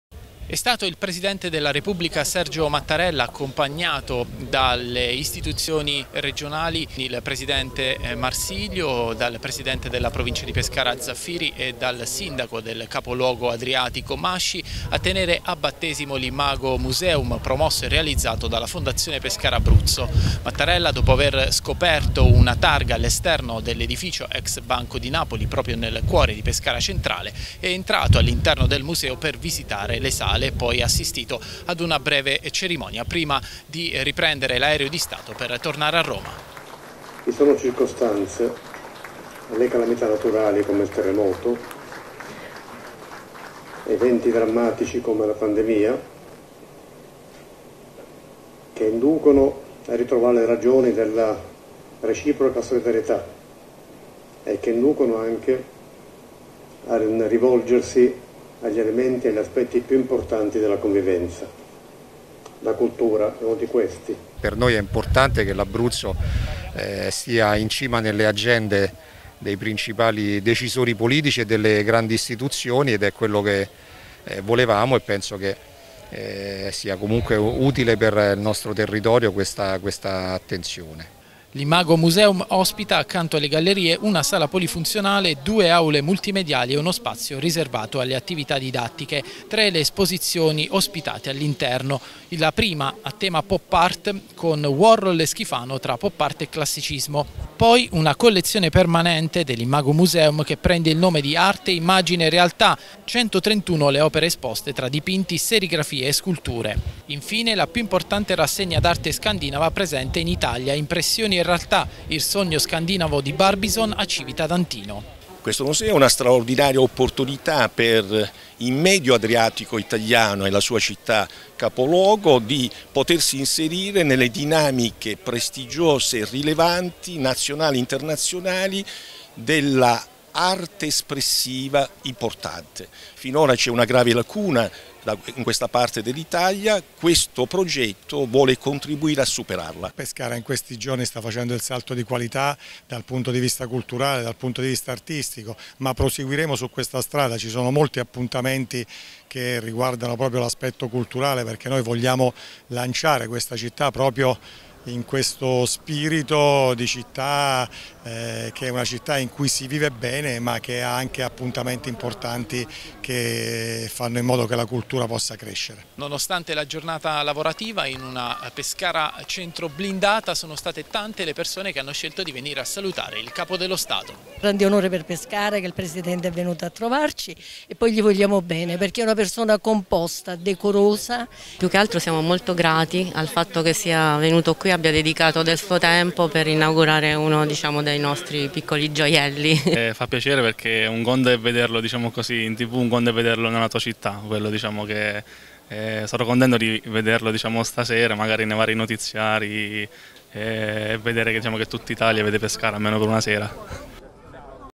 Mattarella inaugura l'Imago Museum
Mattinata speciale per Pescara che ha ospitato il Presidente della Repubblica Sergio Mattarella, arrivato in riva all'Adriatico per inaugurare l'Imago Museum voluto dalla Fondazione Pescarabruzzo. Il Capo dello Stato, in un breve discorso, ha sottolineato il valore della cultura anche e soprattutto nei momenti più difficili di una comunità